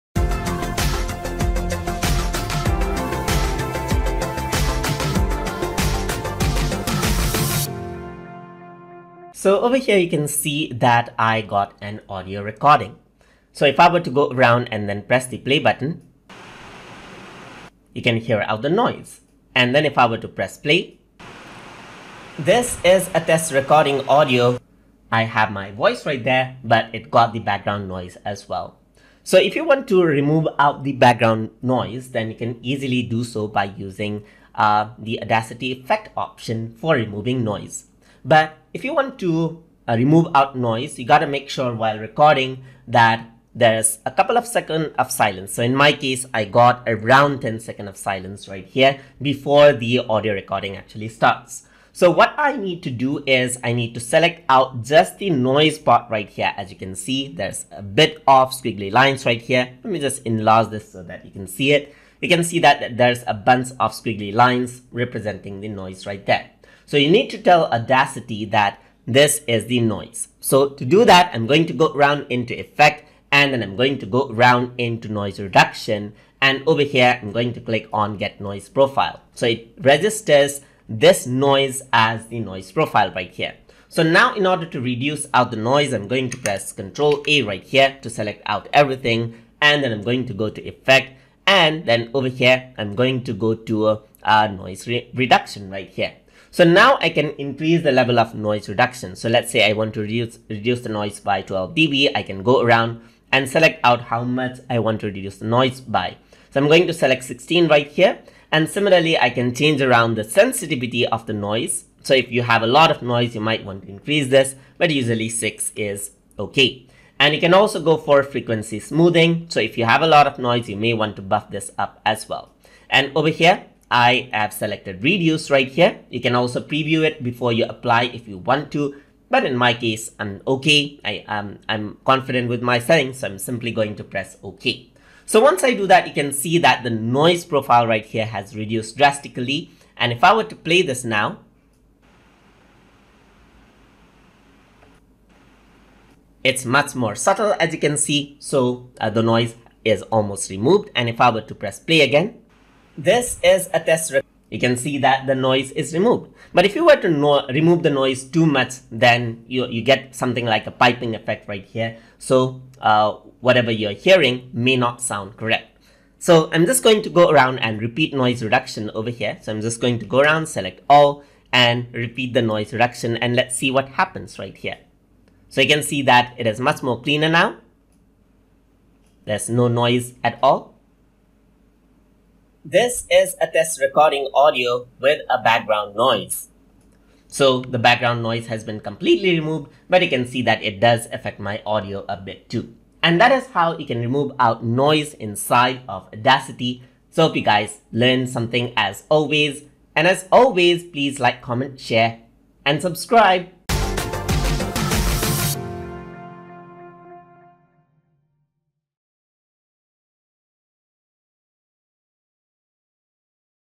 Audacity Tutorial - Lesson 37 - Removing Noise
In this tutorial, we will be discussing about Removing Noise in Audacity